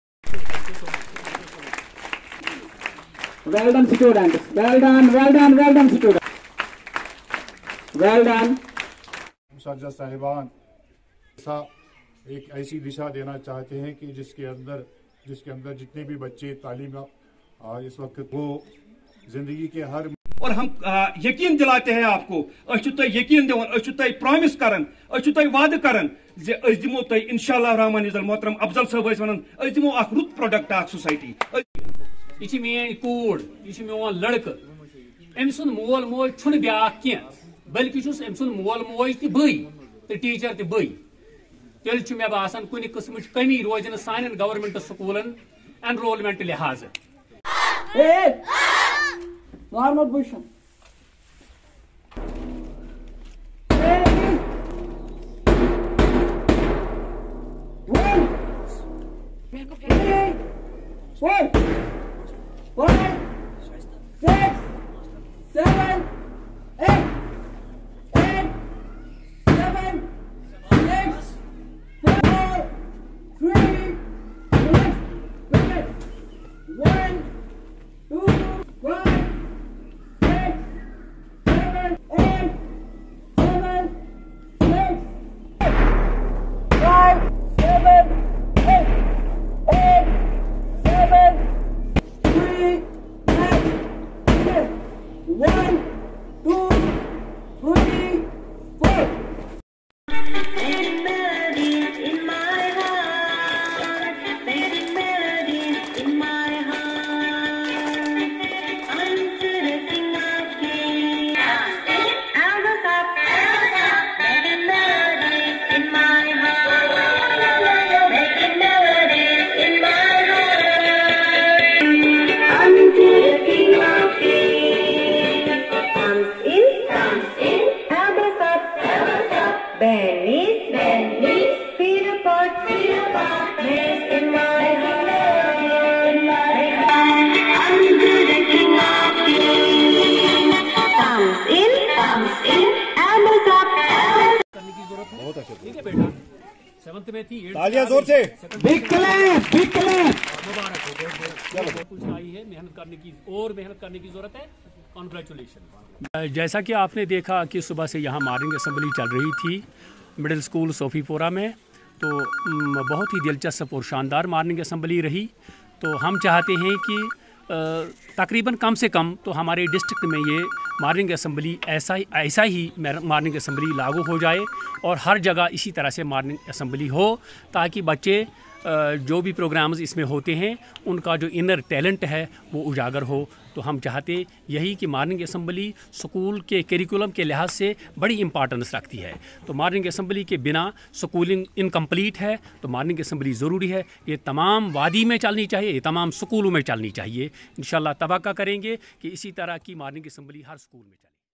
Demonstration program of school morning assembly. held at govt middle school Sofi pora Kulgam#
Demonstration program of school morning assembly. held at govt middle school Sofi pora Kulgam Deputy Chief Education officer Ab Gani was chief guest#